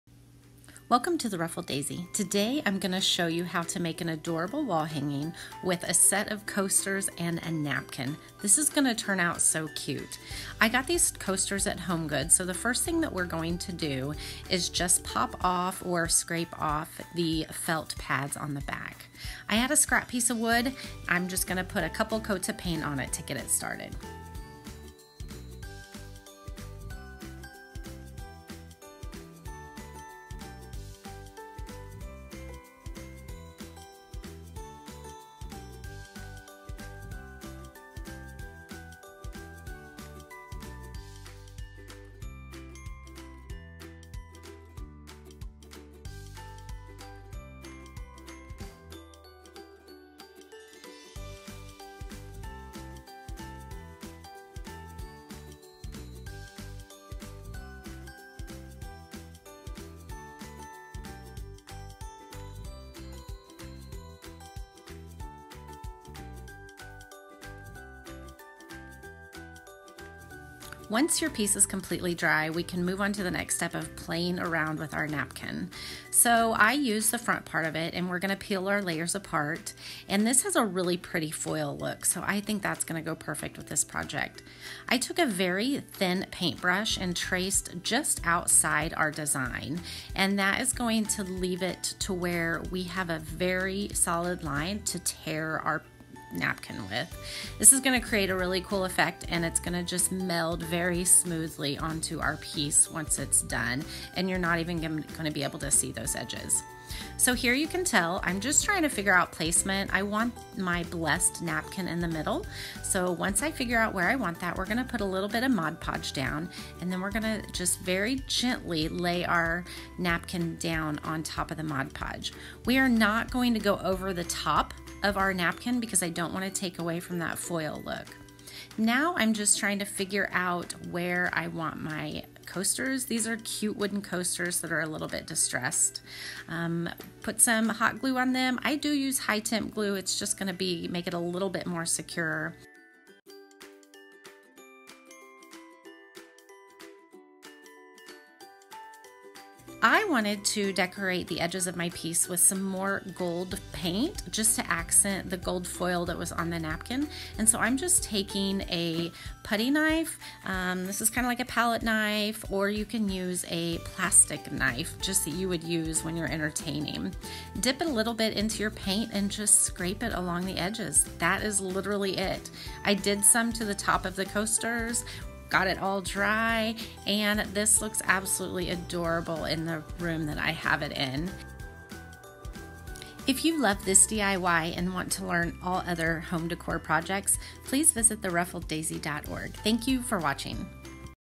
How to Make Stunning DIY Coasters with Napkins | Budget-Friendly Home Decor
Transform scrap wood, coasters, and napkins into elegant home decor! ✨ Watch this easy DIY tutorial and create a stunning, budget-friendly craft that will amaze you! 🎨 How to Make Stunning DIY Coasters with Napkins. Budget-Friendly Home Decor.
Join me as I share how to create an adorable and elegant home decor piece using some scrap wood, drink coasters and a napkin!  You're not going to believe that this project turns out the way it does!  So elegant!

For all things DIY, recipes and inspiration, visit my

Love budget-friendly DIY crafts? 🎨✨ Join me as I transform scrap wood, drink coasters, and a simple napkin into an elegant home decor piece you won’t believe! This easy, fun project is perfect for beginner and expert crafters alike.

✔️ Upcycle materials into something beautiful
✔️ Easy step-by-step tutorial for stunning results
✔️ Budget-friendly DIY home decor idea

You'll be amazed at how simple materials can create such an elegant look! Whether you're making them for yourself or as a handmade gift, these DIY coasters will impress.